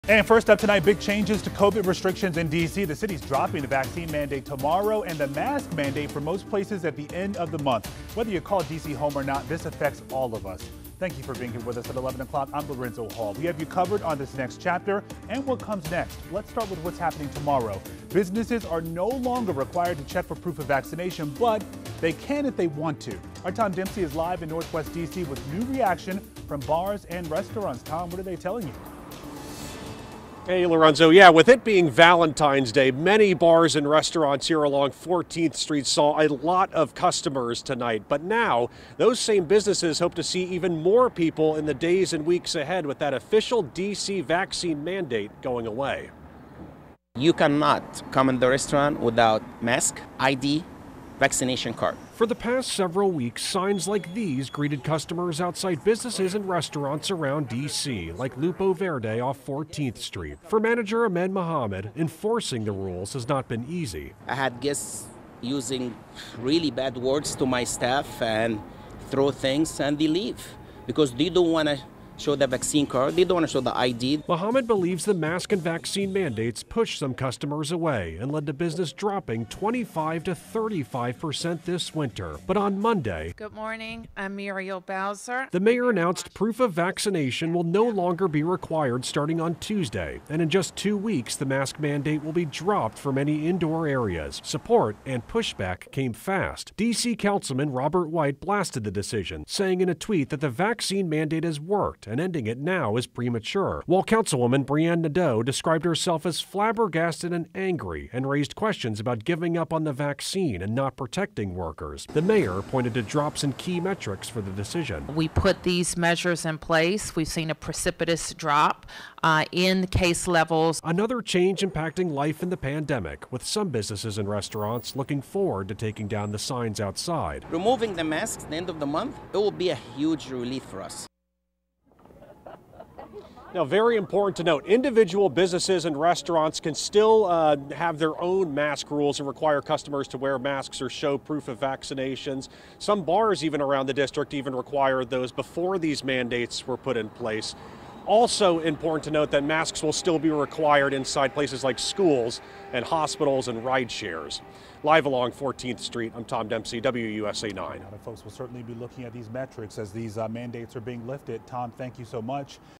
Proof of vaccination and mask mandates ending in most of DC
Mayor Bowser says businesses will no longer have to verify vaccination status for customers on Feb, 15, and mask mandates at most indoor facilities end Feb. 28.